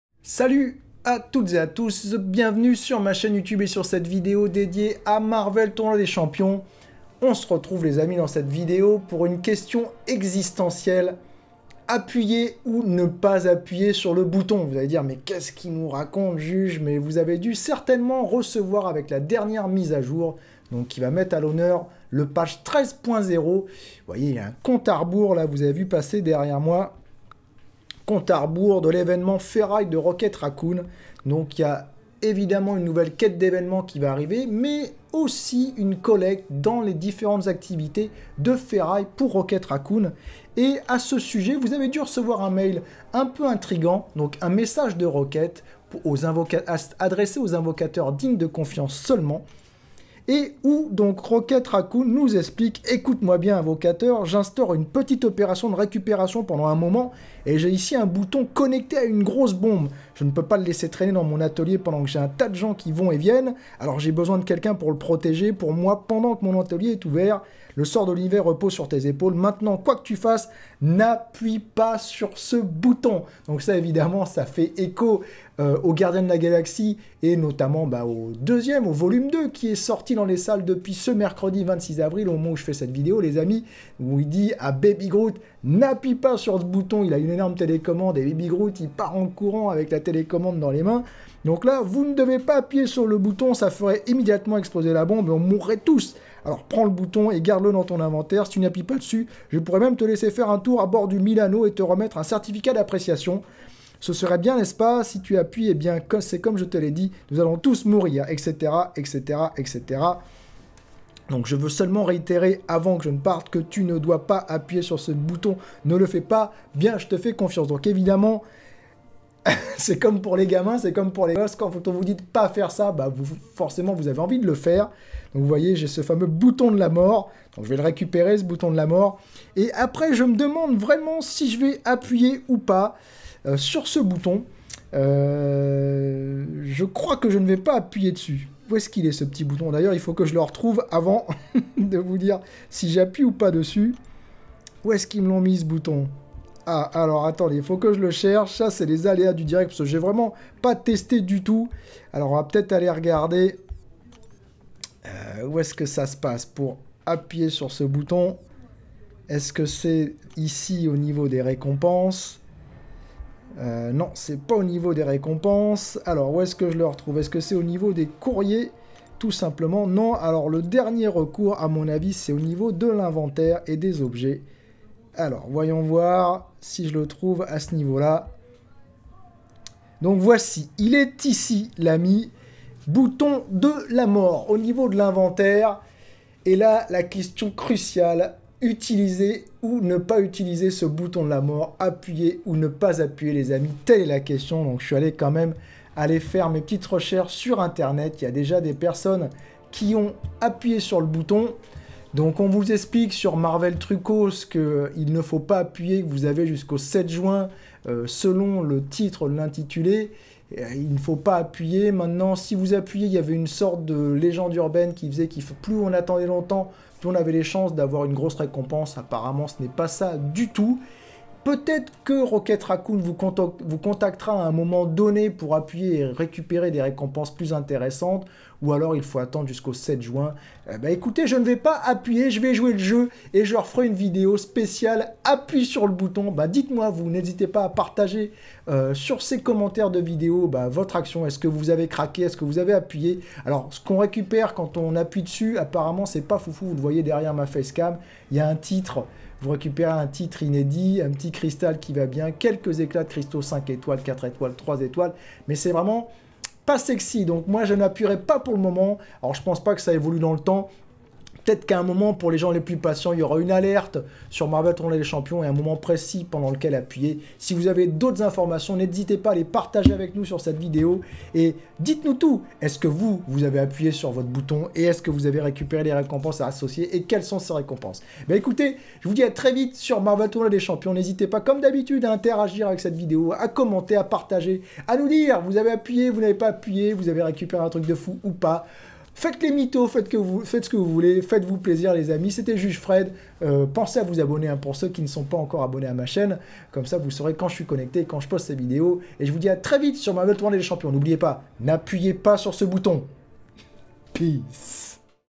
Événement BOUTON DE LA MORT - N'APPUYEZ PAS !!!
Hello les Invocateurs ! Rocket Raccoon vous a confié le BOUTON DE LA MORT. Vous avez appuyé ? PEACE ! ^^